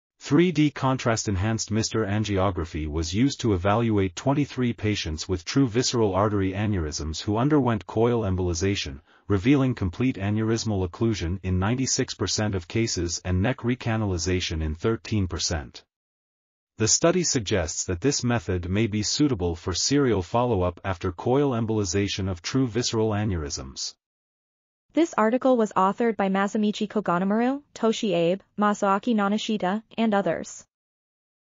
Follow-up of true visceral artery aneurysm after coil embolization by three-dimension... | RTCL.TV
### Keywords ###

### Article Attribution ###
Title: Follow-up of true visceral artery aneurysm after coil embolization by three-dimensional contrast-enhanced MR angiography
Authors: Masamichi Koganemaru, Toshi Abe, Masaaki Nonoshita, Ryoji Iwamoto, Masashi Kusumoto, Asako Kuhara ,and Tomoko Kugiyama
Publisher: Galenos Publishing House
DOI: 10.5152/dir.2013.13236

### Video Timestamps ###
0:00:00 - Summary
0:00:24 - Title
0:00:31 - End